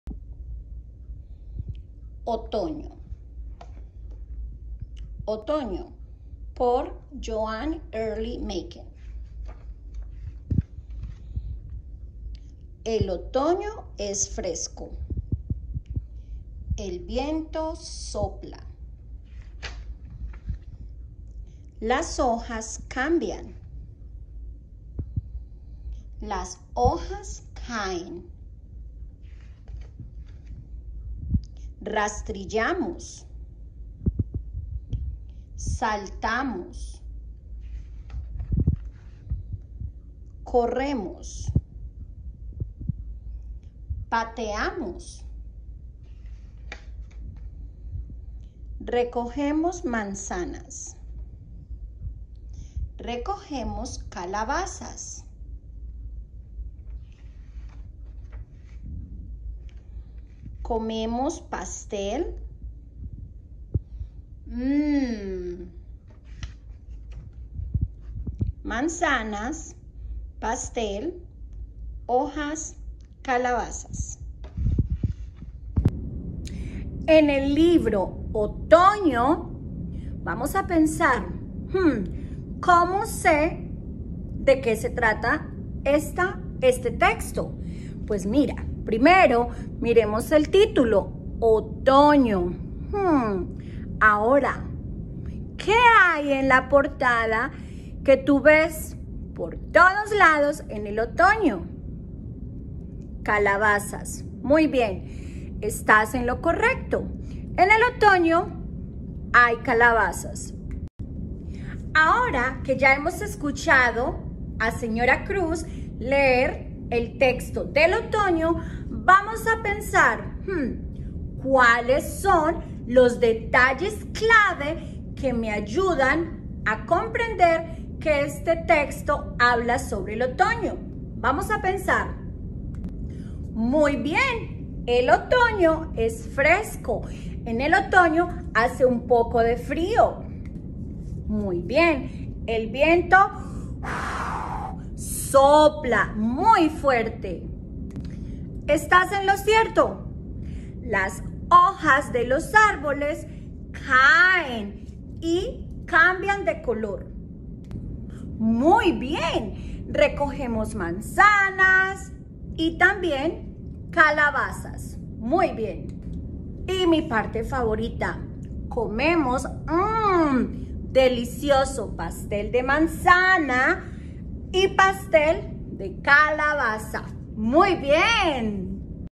Tema principal y detalles clave del texto/ main topic and key details of the text.
Aprendamos a identificar el tema principal y las ideas clave del texto.